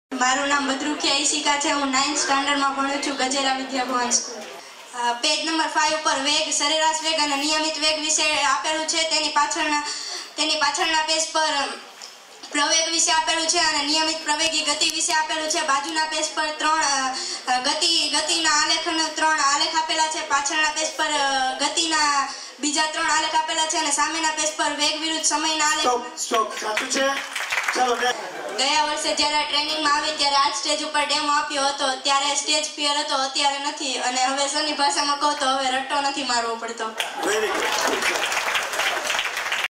Demo After Dharmesh Pithva's Memory Training By Ishika Badrukiya Surat 2017
Dharmesh Pithva's Magic of Memory  Workshop @ surat-2017

1.Gandhi smruti bhavan-surat

2.Sardar smruti bhavan -surat

3. Sanjivkumar auditorium-surat

4. APMC krushi bazar auditorium-surat

5. Jivanbharti school auditorium-surat